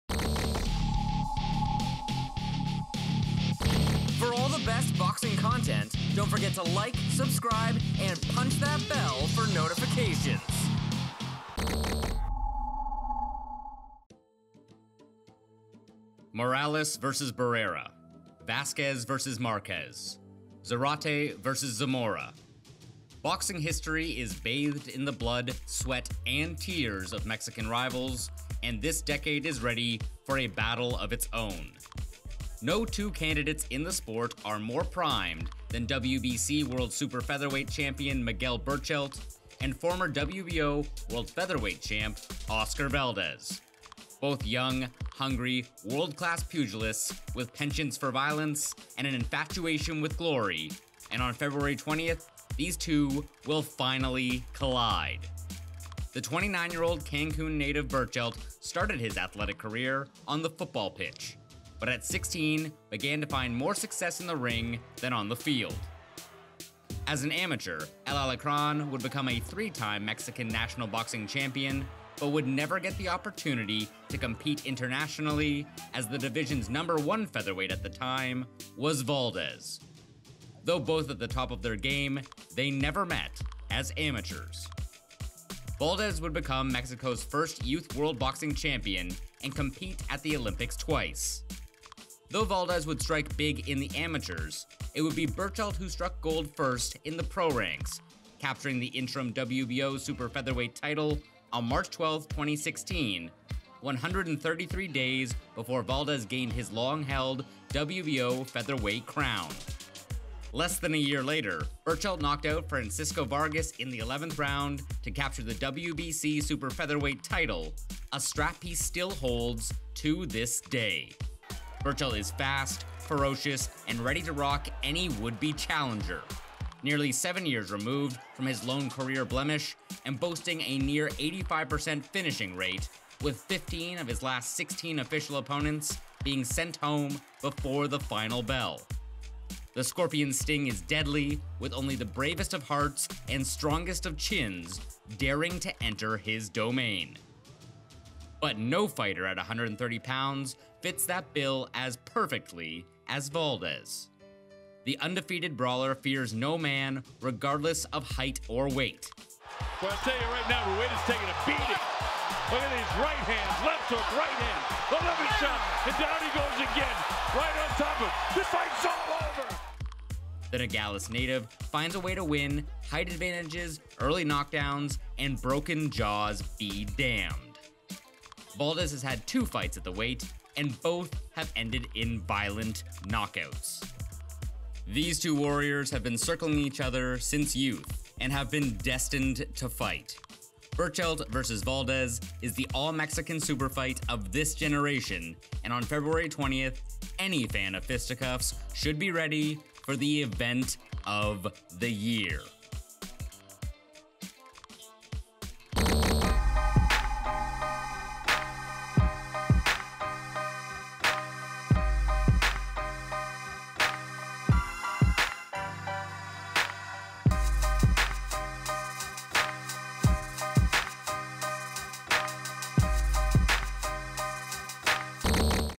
MIGUEL BERCHELT vs. OSCAR VALDEZ CHAMPIONSHIP PREVIEW I Feature, Highlights I BOXING WORLD WEEKLY
On February 20, 2021, the next great instalment of all-Mexican match ups will take place between Miguel Berchelt and Oscar Valdez!

These two have been on a war path since their amateur days and will finally meet for Berchelt's (37-1, 33 KOs) WBC World Super Featherweight Title. This will be 'El Alacron's" sixth title defence and it's against the uber talented Valdez (28-0, 22 KOs) in The Bubble in Las Vegas, USA.

These two are arguably the best the 130-lb. division has to offer, and with the added fuel of being compatriots, it's the very definition of appointment viewing.